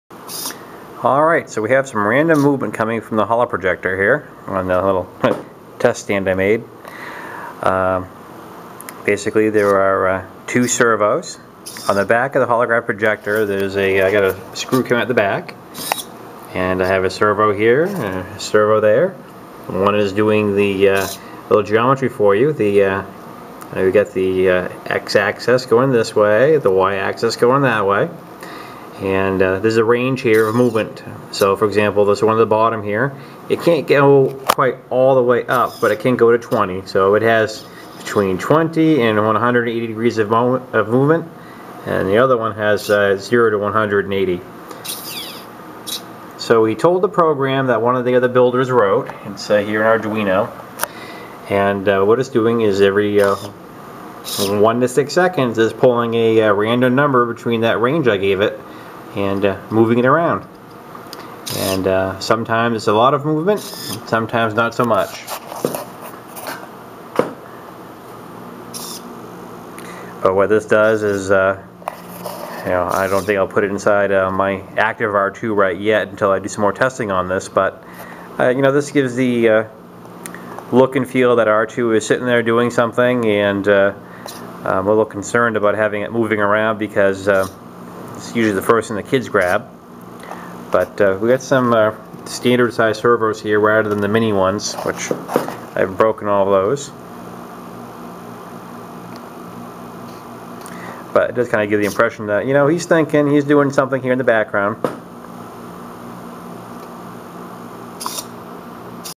Trying out some random Holoprojector movement
Using two standard sized (ball bearing) servos so that the holoprojector can randomly move around.  Another R2-D2 Builder wrote the code for the Arduino to randomly move the servos around.  The builder enters in the range of motion for each servo along the X and Y axis.  The hard part is finding out what your servo range is, from 0 to 180.  I recommend a Servo Tester which will determine the neutral position (middle).  That
s 90, then from there, you have to determine how far your servo and linkage can move. Once you have established what your range is, this is how it all looks. Its working pretty well but we will see how well all of this mounts into the dome. and with the holoprojector being the first thing the kids grab, have to see about accounting for that. ("Don't touch" doesn't seem to work these days. )